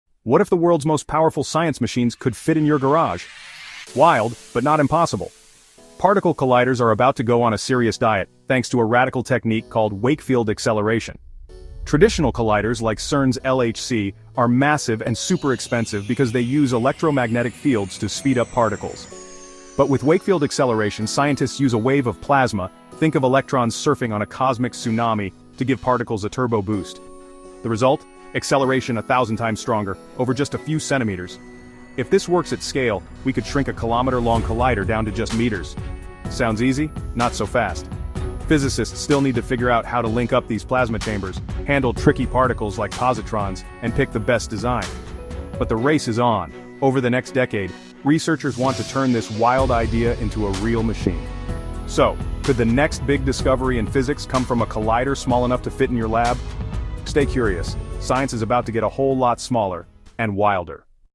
Wakefield Accelerator: The Future of Particle Acceleration
Explore the science behind Wakefield accelerators — a revolutionary technology using plasma waves to accelerate particles to near light-speed in compact setups, promising faster, smaller, and more efficient particle accelerators.